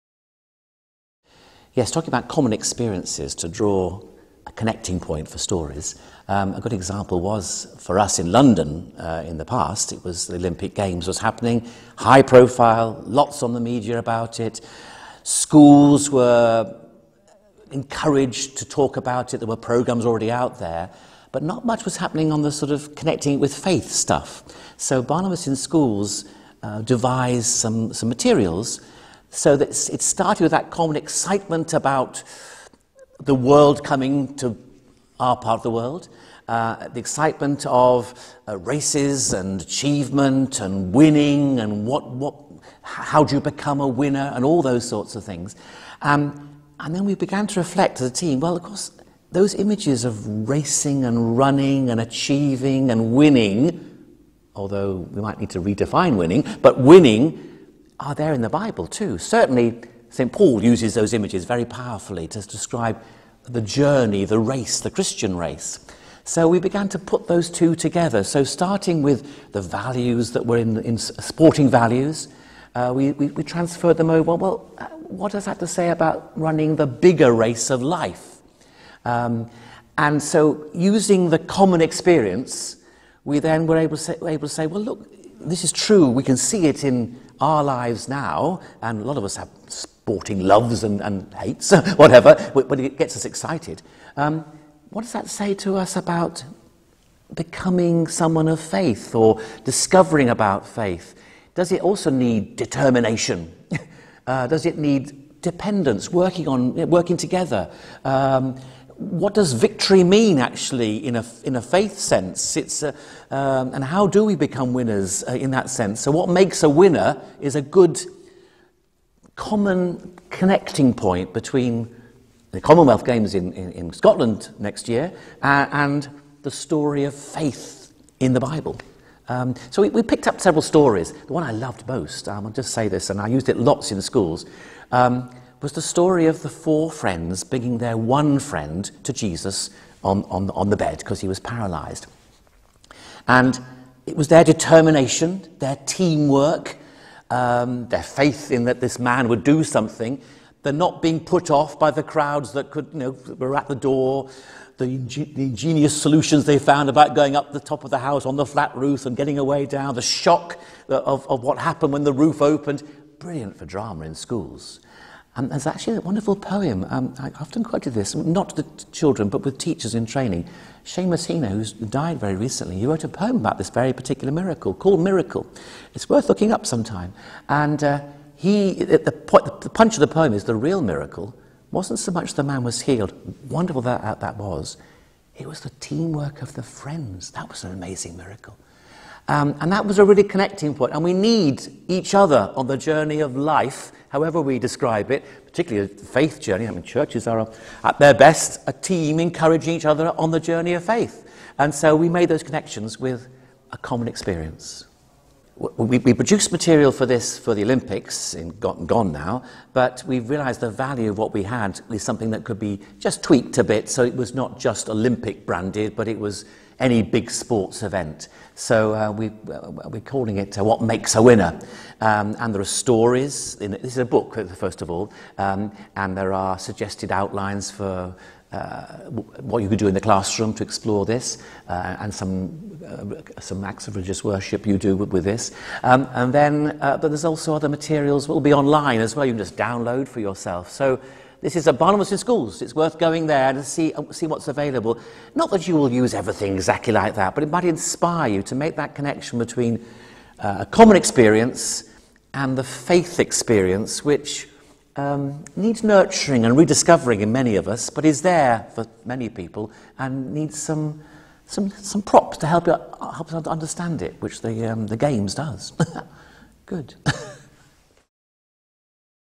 School and Church Resources for the Commonwealth Games
Martyn Payne, who works with Barnabas explains how lessons learned from using material developed for the London Olympics has informed its growth into a broader resource, ideally suited for making connections between sport and principles of faith, and easily available for use in churches and schools ahead of the Commonwealth Games in Glasgow, 2014.